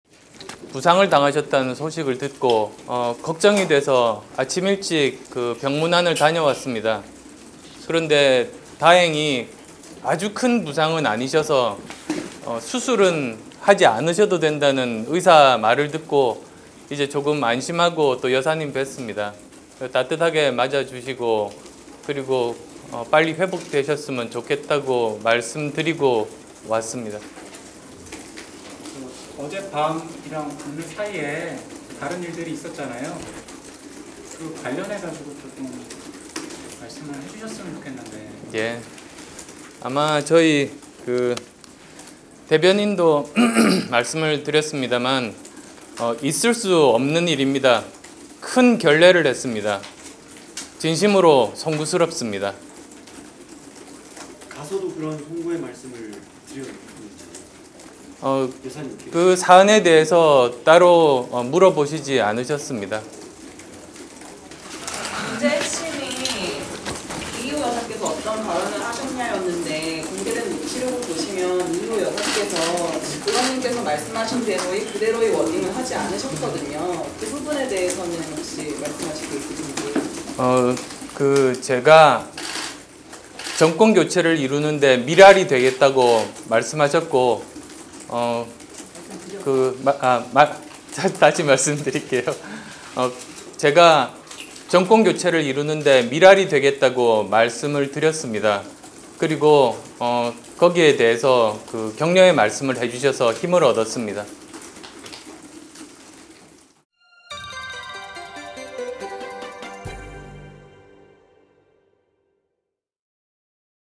안철수 "큰 결례", 이희호 여사 방문 녹취록 공개 파문 사과
안철수 국민의당 의원이 27일 오전 고 김대중 대통령 부인 이희호 여사와의 지난 4일 비공개 대화 녹취가 유출된 데 대해 공식 사과했다.

안철수 의원은 27일 오전 낙상으로 입원한 이희호 여사를 문병하고 "있을수 없는 일이다, 큰 결례를 했다"며 직접 사과의 뜻을 전했다고 밝혔다.

(촬영·편집 - 강신우 기자)